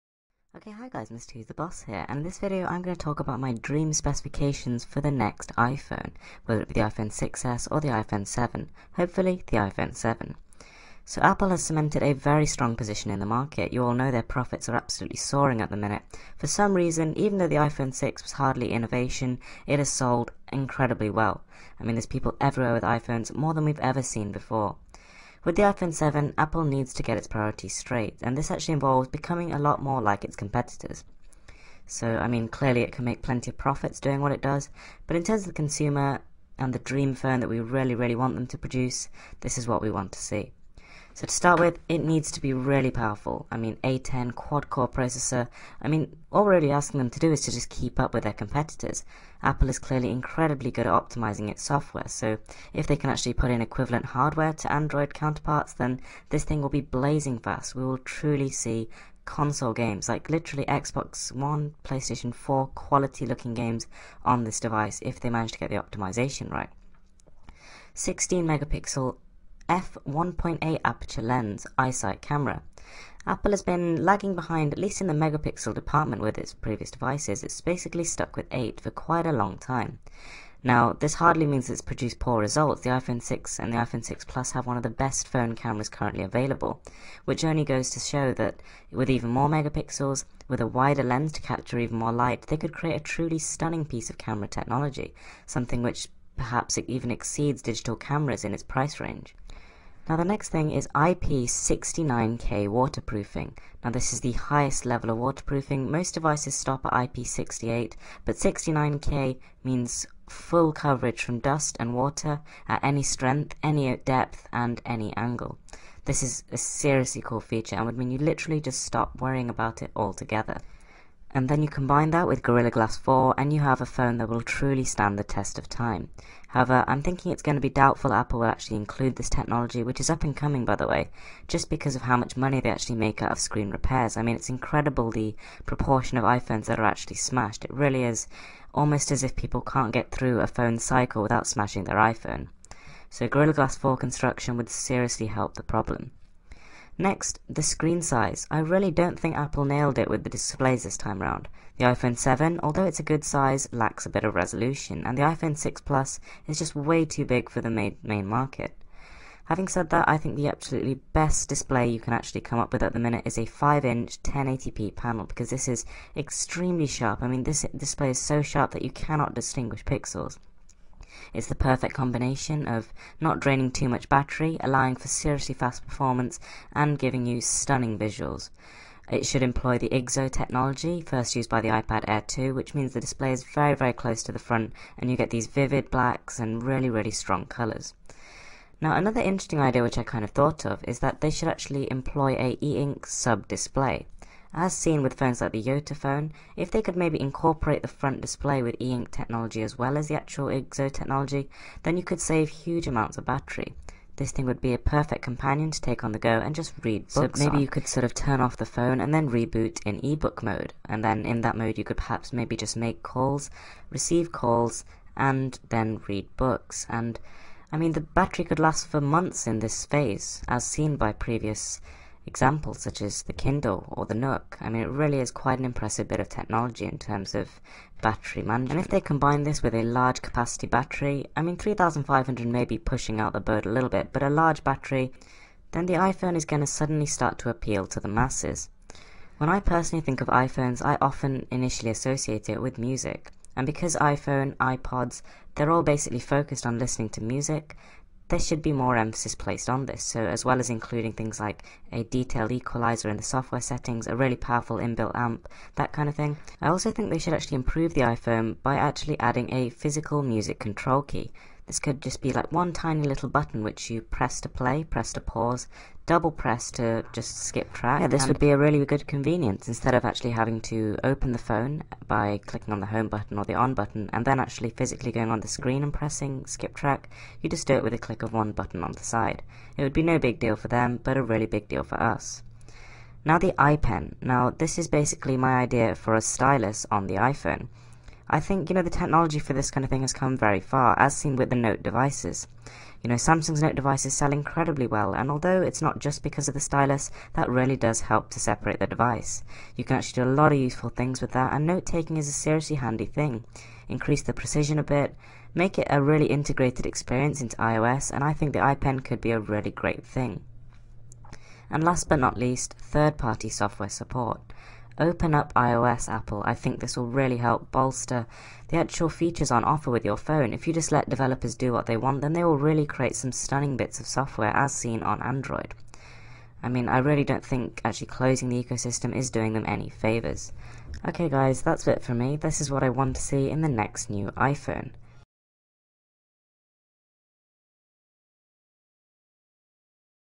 IPhone 6S and IPhone 7 Features
IPHONE 7
A10 Quad Core 2.5GHz
3GB Ram
16MP f1.8 iSight Camera
IP69K Waterproofing
Gorilla Glass 4
5.0" 1080p IGZO Display
E-Ink Sub Display
3500mAh Battery
Music Control Keys
iPen
3rd Party Software Support